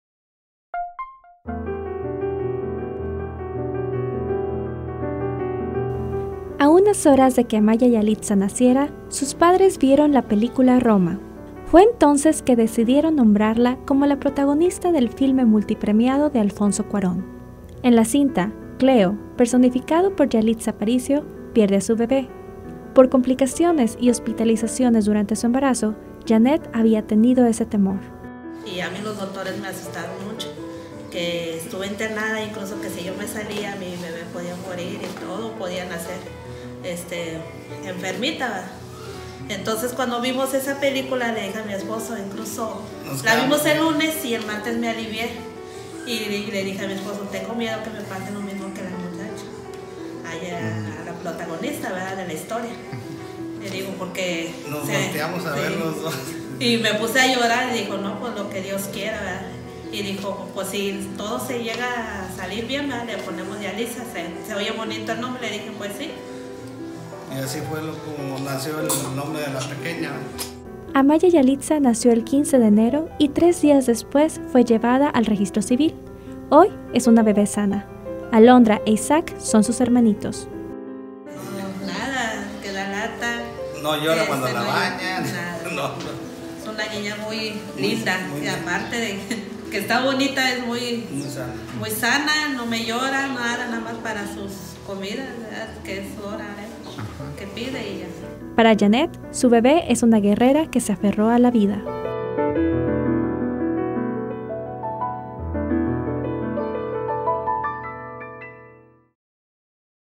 Nombran Yalitza a bebé regia
Una pareja llama a su bebé Yalitza debido a que la madre sufrió complicaciones en el embarazo, como el personaje de Aparicio en la película "Roma".